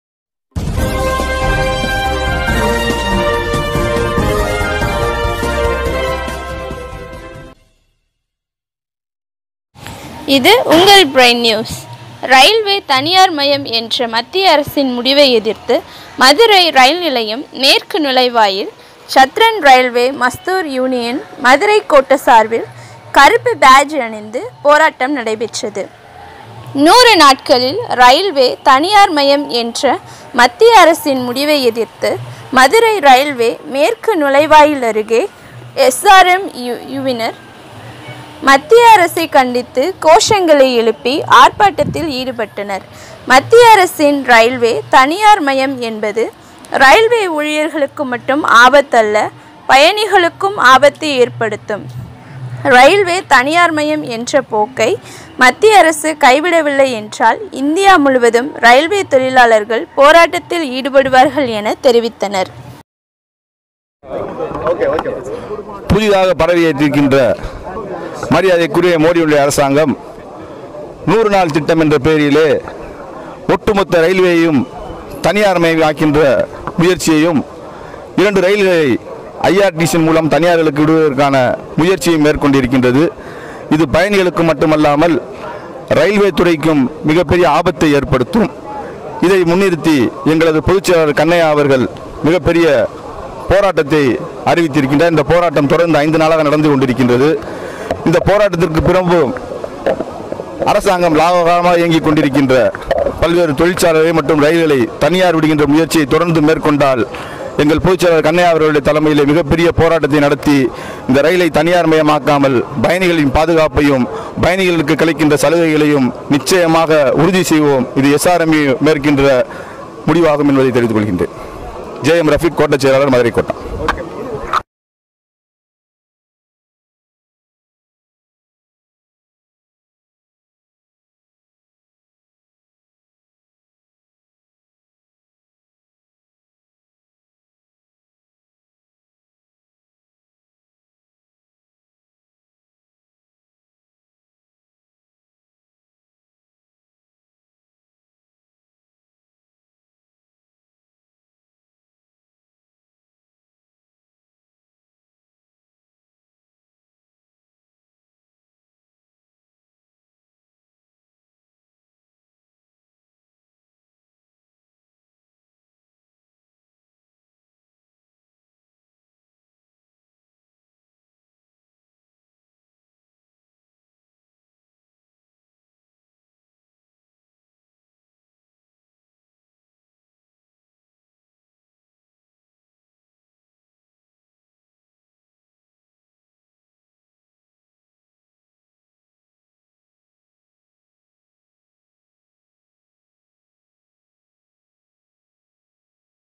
ரயில்வே தனியார்மயம் என்ற மத்திய அரசின் முடிவை எதிர்த்து மதுரை ரயில் நுழைவாயில் போராட்டம் நடைபெற்றது
ரயில்வே தனியார்மயம் என்ற மத்திய அரசின் முடிவை எதிர்த்து மதுரை ரயில் நிலையம் மேற்கு நுழைவாயில் சதர்ன் ரயில்வே மஸ்தூர் யூனியன் மதுரை கோட்ட சார்பில் கருப்பு பேட்ஜ் அணிந்து போராட்டம் நடைபெற்றது

100 நாட்களில் இரயில்வே தனியார் மயம் என்ற மத்திய அரசின் முடிவை எதிர்த்து மதுரை இரயில்வே மேற்கு நுழைவாயில் அருகே எஸ்.ஆர்.எம்.யுவினர் மத்திய அரசை கண்டித்து கோசங்களை எழுப்பி ஆர்ப்பாட்டத்தில் ஈடுபட்டனர், மத்திய அரசின் ரயில்வே தனியார் மயம் என்பது இரயில்வே ஊழியர்களுக்கு மட்டும் ஆபத்தல்ல , பயணிகளுக்கும் ஆபத்தை ஏற்படுத்தும், இரயில்வே தனியார் மயம் என்ற போக்கை மத்திய அரசு கைவிடவில்லை என்றால் இந்தியா முழுவதும் இரயில்வே தொழிலாளர்கள் போராட்டத்தில் ஈடுபடுவார்கள் என தெரிவித்தனர்.